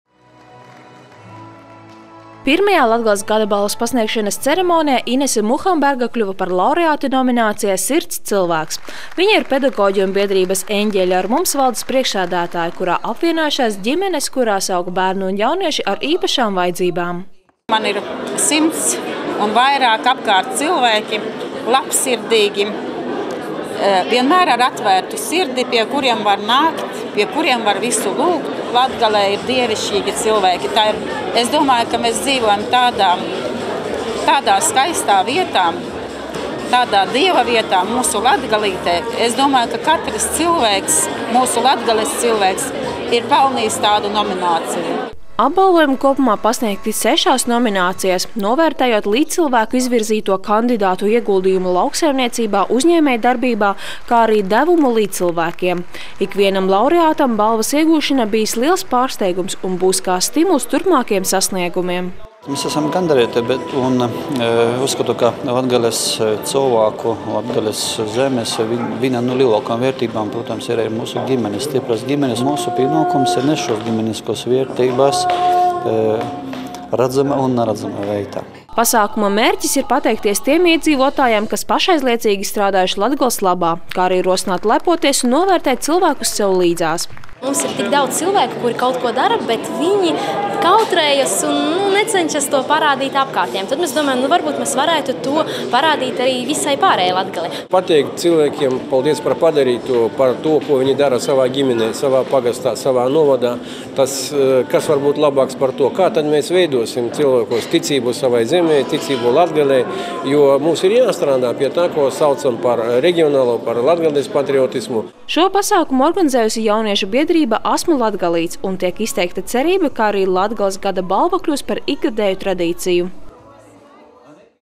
Pirmo reizi pasniegta Latgales Gada balva
Pirmo reizi pasniegta Latgales Gada balva, kurai paši Latgales cilvēki izvirzījuši tos 35 kandidātus, kas viņuprāt būtu pelnījuši atzinību 6 nominācijās- lauksaimniecībā, uzņēmējdarbībā, gada seniors, jaunietis, kā arī sirds cilvēks un ģimene. Iegūtais apbalvojums laureātiem nācis negaidīti, jo viņi visu darījuši savu līdzcilvēku labā.